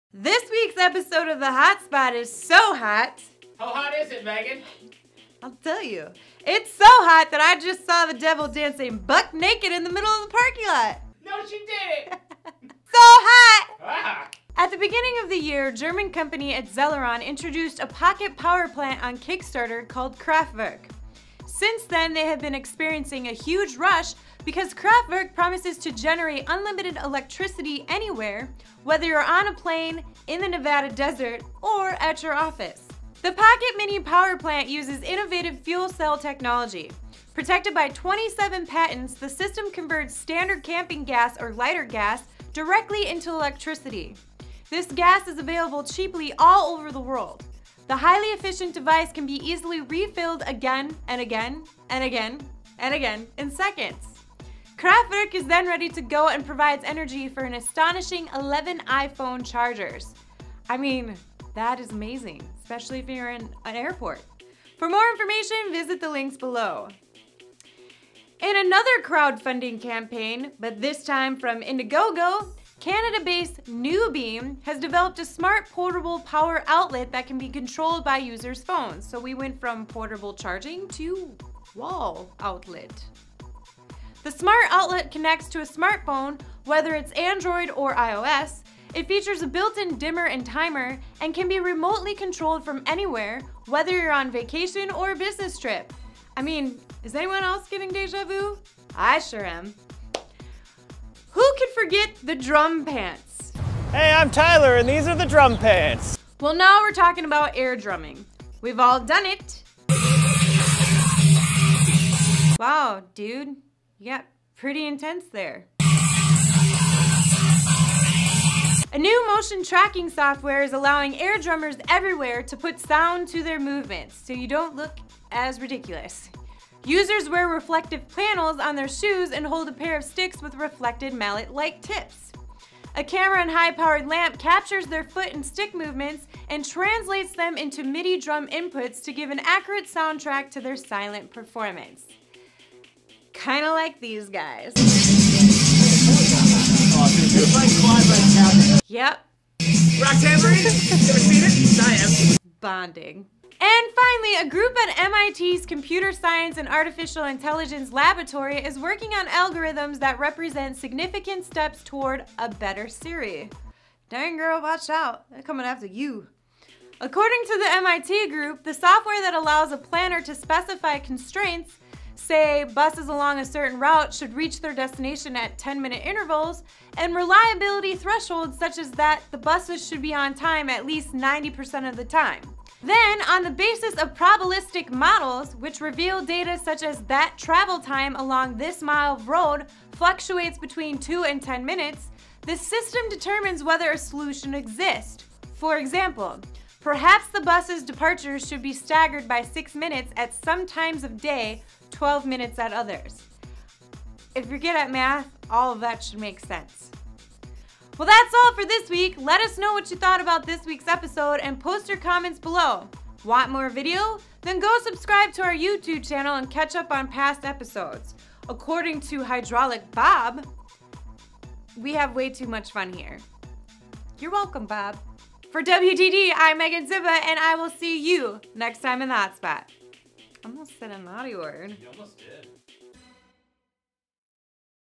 HotSpot Episode 98: MIT Researchers Create a Better Siri
This week on WDD’s HotSpot:

--- Portable Power Plant

At the beginning of the year, German company, eZelleron introduced a pocket power plant on kickstarter called kraftwerk. Since then, they have been experiencing a huge rush, because kraftwerk promises to generate unlimited electricity anywhere, whether you’re on a plane, in the Nevada desert, or at your office. The pocket mini-power plant, uses innovative fuel cell technology. Protected by 27 patents, the system converts standard camping gas or lighter gas directly into electricity. This gas is available cheaply all over the world. The highly efficient device can be easily refilled again and again, in seconds. kraftwerk is then ready to go and provides energy for an astonishing 11 iPhone charges.

--- Smart Outlet

Canada-based NewBeem, has developed a smart portable power outlet that can be controlled by users’ phones. The smart outlet connects to a smartphone, whether it’s Android or iOS. It features a built-in dimmer and timer, and can be remotely controlled from anywhere, whether you’re on vacation or a business trip.

--- Air Drumming

A new motion tracking software is allowing airdrummers everywhere to put sound to their movements. Users wear reflective panels on their shoes and hold a pair of sticks with reflective mallet-like tips. A camera and high-powered lamp captures their foot and stick movements, and translates them with into midi drum inputs to give an accurate soundtrack to their silent performance.

--- A Better Siri

A group at MIT’s Computer Science and Artificial Intelligence Laboratory is working on algorithms that represent significant steps toward “a better Siri”. According to the MIT group, the software that allows a planner to specify constraints — say, buses along a certain route should reach their destination at 10-minute intervals — and reliability thresholds, such as that the buses should be on time at least 90 percent of the time. Then, on the basis of probabilistic models — which reveal data such as that travel time along this mile of road fluctuates between two and 10 minutes — the system determines whether a solution exists: For example, perhaps the buses’ departures should be staggered by six minutes at some times of day, 12 minutes at others.